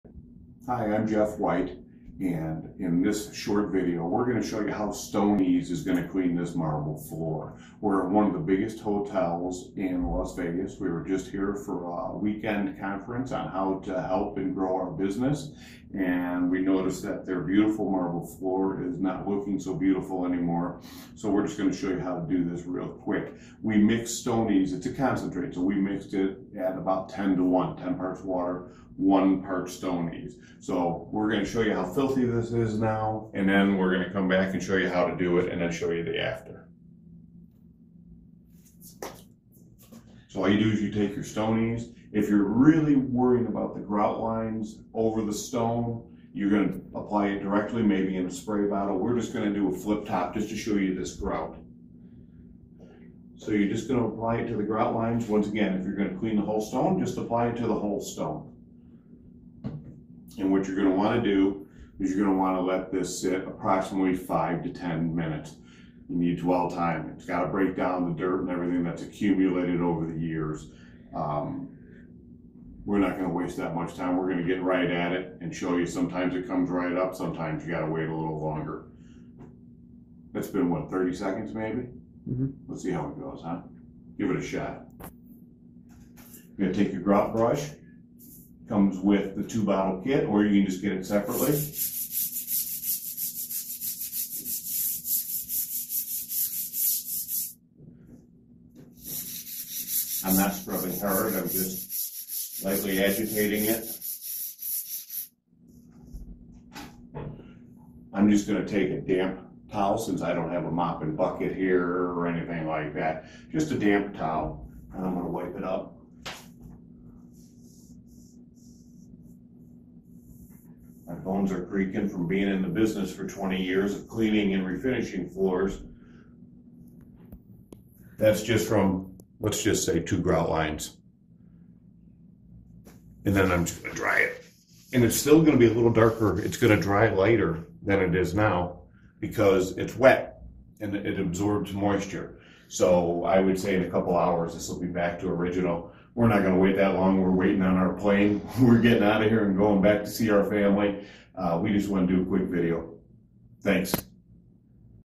Cleaning a Marble Floor In Las Vegas Hotel Bathroom
In this quick video we demonstrate  how Stone-eez works on a marble floor. We were in Las Vegas for a class on how to grow our business and decided why not shoot a video cleaning the floor and show everyone how easy it can be!